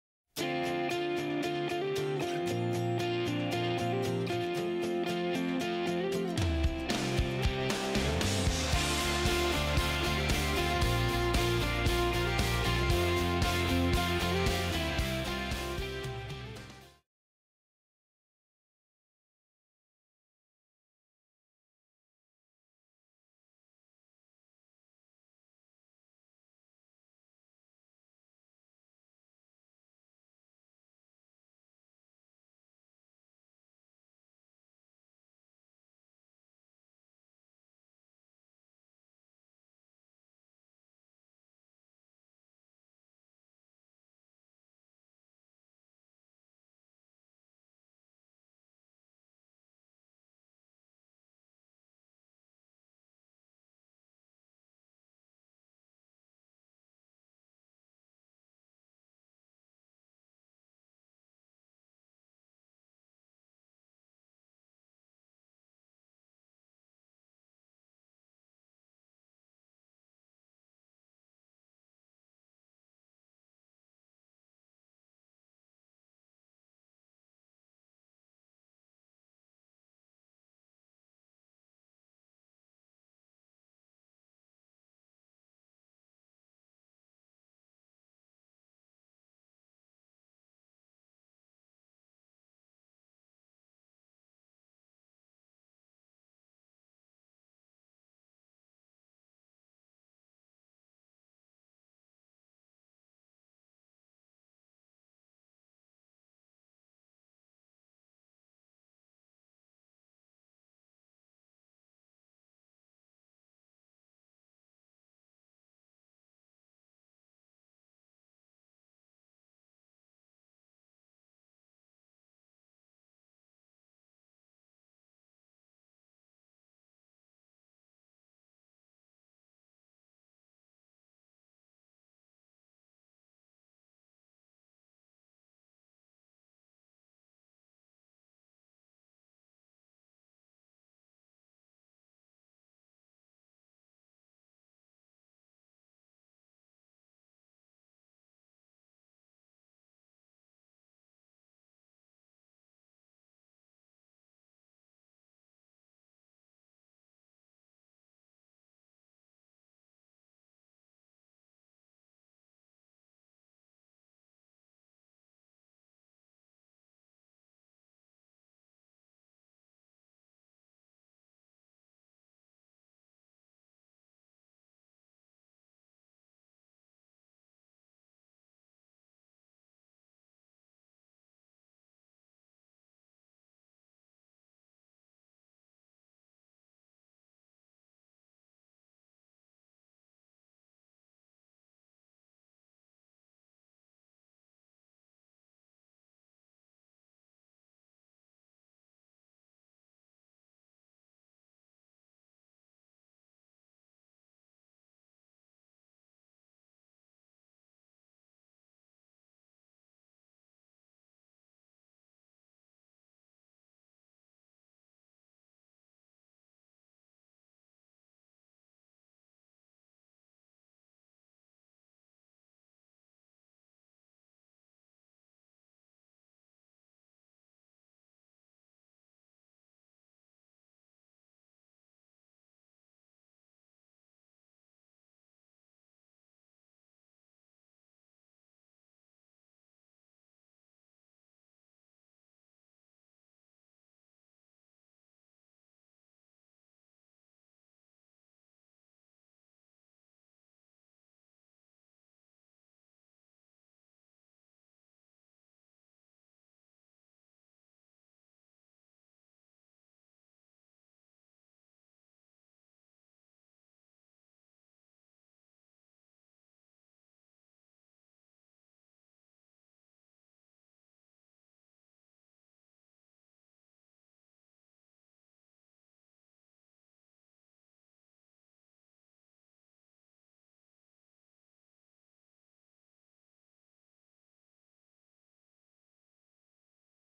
[슈퍼애니멀로얄 모작]쥬신게임아카데미 게임프로그래밍학원 게임프로그래머 5개월차 Direct2D 개인작품 시연회_95기 장혜영_툴
쥬신게임아카데미 게임프로그래밍학원 게임프로그래머 5개월차 Direct2D 개인작품 시연회_95기 장혜영_툴